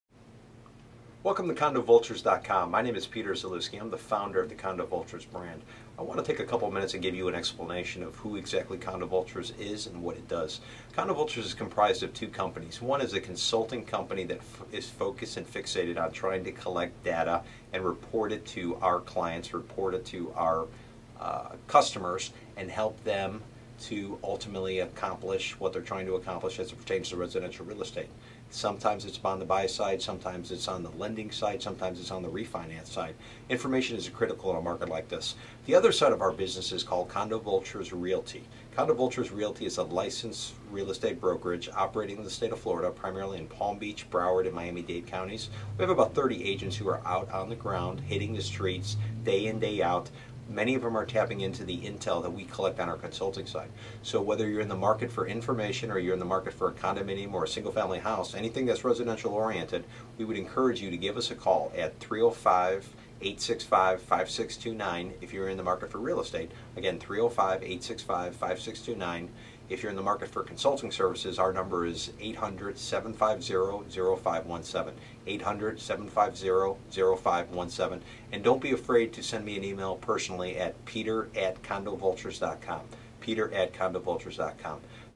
Condo Vultures® Founder Peter Zalewski Discusses His Vision - Condo Vultures®
Relying on hard-nosed financial journalists and veteran analysts, Condo Vultures® has established itself as a vital intelligence source to assist in evaluating the state of the residential real estate market of South Florida.